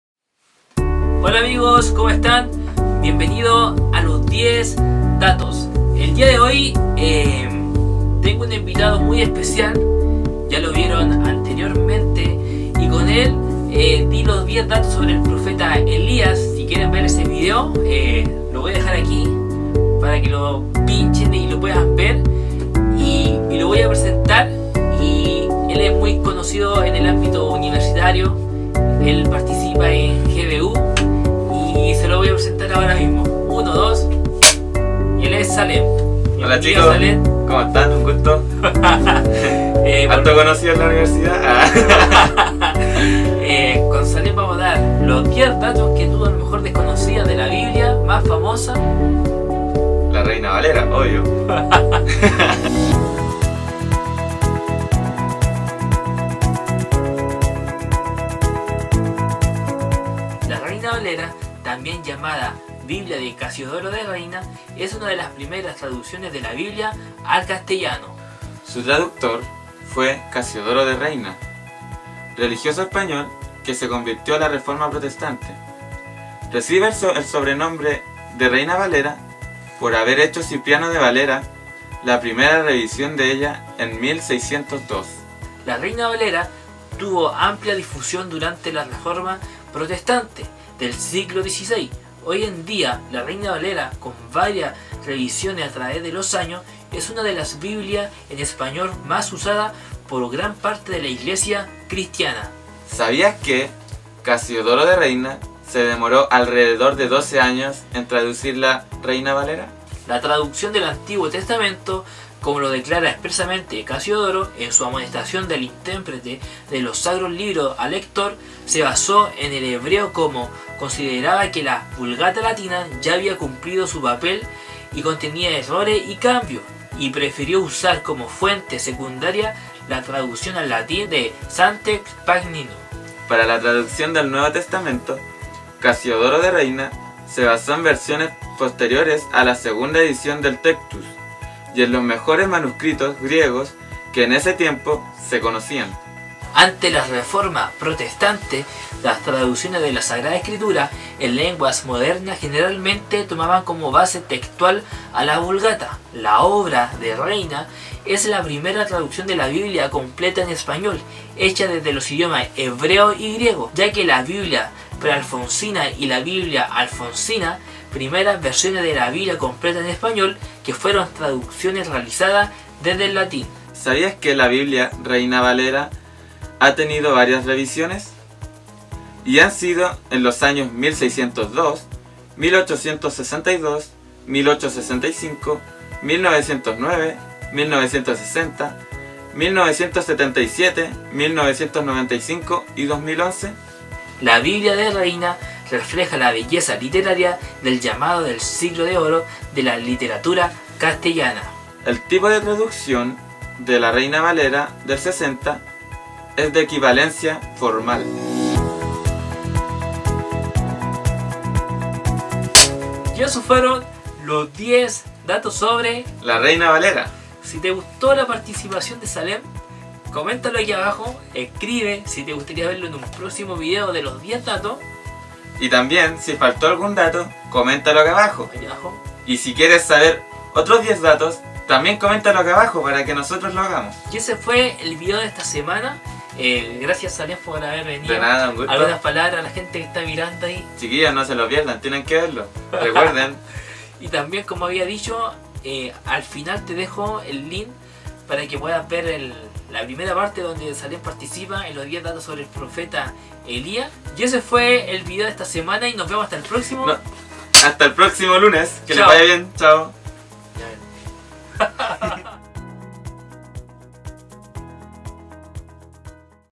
10 Datos sobre: La Biblia Reina valera
Disfruta de nuestro nuevo bloque titulado: 10 Datos. donde conoceremos y aprenderemos de los personajes de la Biblia, Dios y la historia en poco tiempo.

Tema de hoy 10 Datos sobre: La biblia Reina Valera